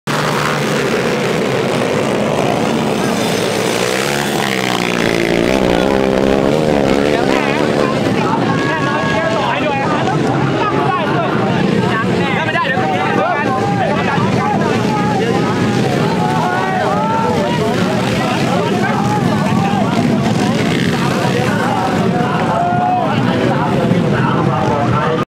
Enduro Off road มะขามเดี่ยวเอ็นดูโร่ครอส อ๊อฟโรดคนรักเฮียการกุศล2557
ร่วมกิจกรรม Enduro Off road มะขามเดี่ยวเอ็นดูโร่ครอส อ๊อฟโรดคนรักเฮียการกุศล ร่วมสมทบทุนเททองหล่อพระประธารอุโบสถ ณ วัดมะขามเดี่ยว จ.ระยอง